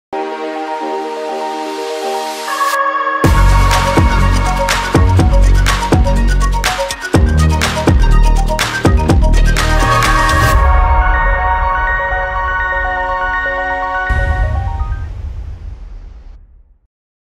Canal Heights de GRISOGONO I DAMAC I Business Bay
A luxurious waterfront living experience that embodies the beauty and perfection of nature's most wondrous creation. the blue pearl. Canal Heights brings natural elements to life with a diverse range of amenities that promise year-round serenity and well-being.

Offering a selection of studios as well as 1 & 2-bedroom apartments in multiple layouts.

Features
✅Multiple domes of intimate dining, Wellness & private parties
✅Underwater pearl museum
✅Shell-shaped swimming pool
✅Infinity pool

Like 👍 Comment 💬 Share 🗣️ and Click on the bell 🔔 icon for the latest updates.